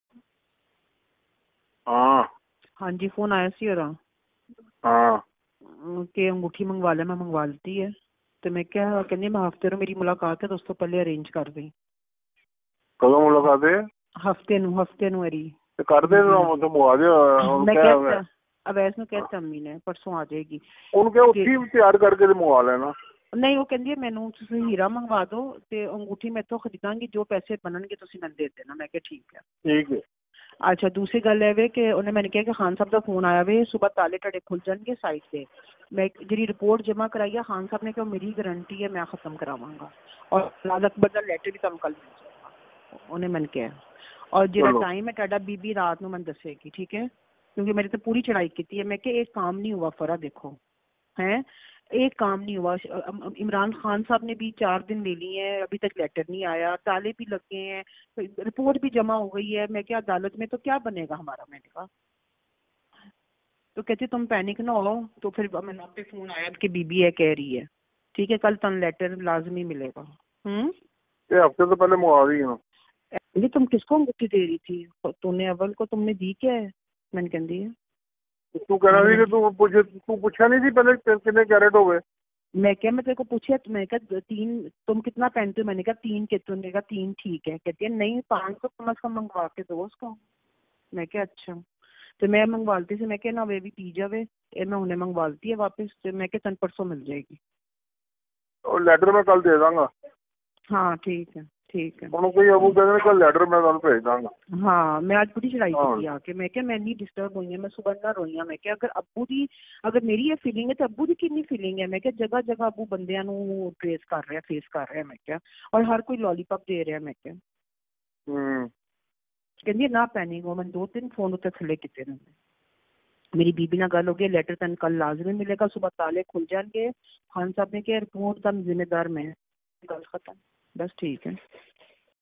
Farah Khan demands diamond gift | New Audio Leak of Farah Khan and Malik Riaz
فرح خان نے ہیرے کا تحفہ مانگ لیا | فرح خان اور ملک ریاض کی نئی آڈیو لیک